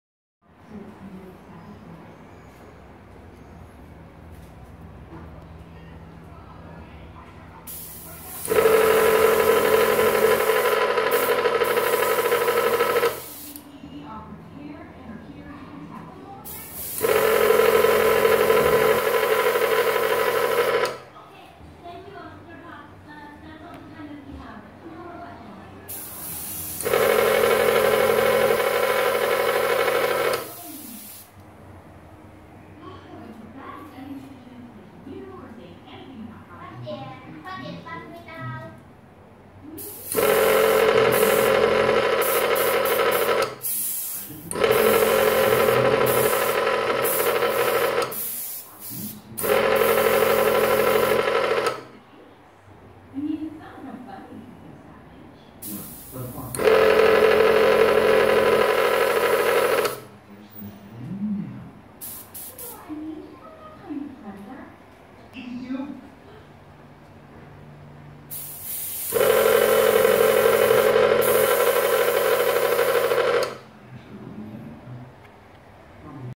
Hao Sheng 浩盛 HS217 airbrush compressor sound test
HS217 AIRBRUSH COMPRESSOR BOUGHT FROM TAOBAO, TRY OUT FOUND IT'S NOT SILENT TYPE.